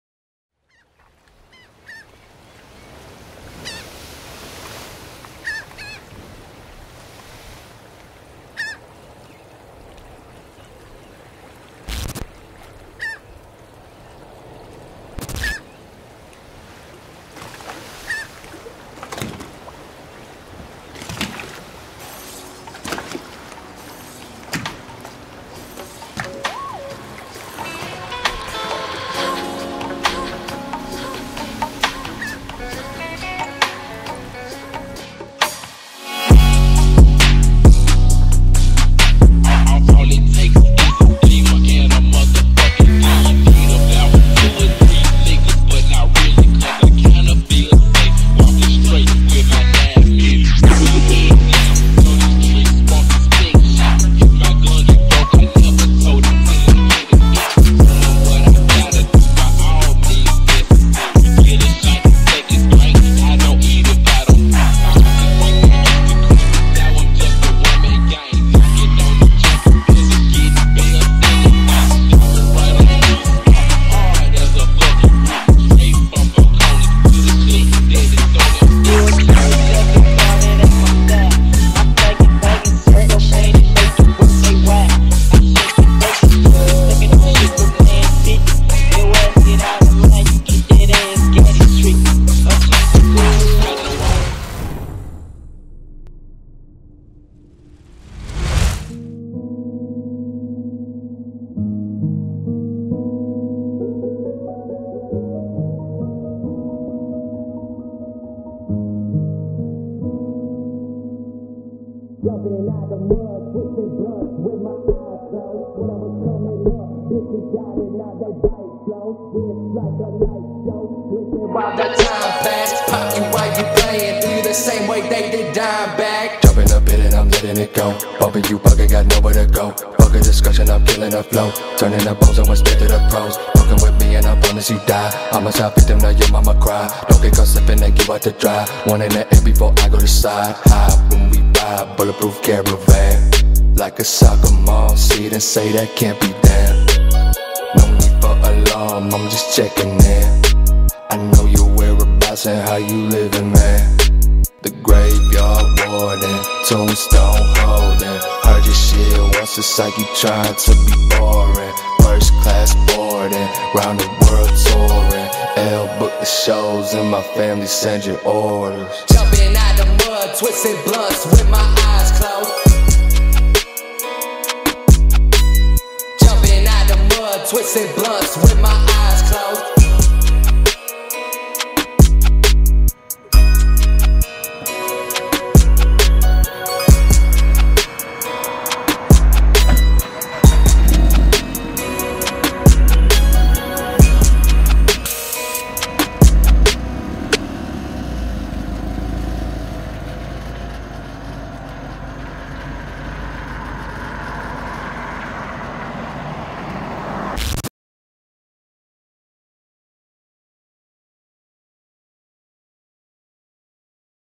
Deviant's Widebody Scion FRS (4K)
I finally had the chance to travel down to Florida and film the beautiful piece of automotive work! Evan has put so much work and dedication into this car which makes this car one hell of a machine! Deviant is a company created by Evan that inspires others to be far from normal. The whole idea behind this build was to be far from normal a.k.a DEVIANT!

SONGS USED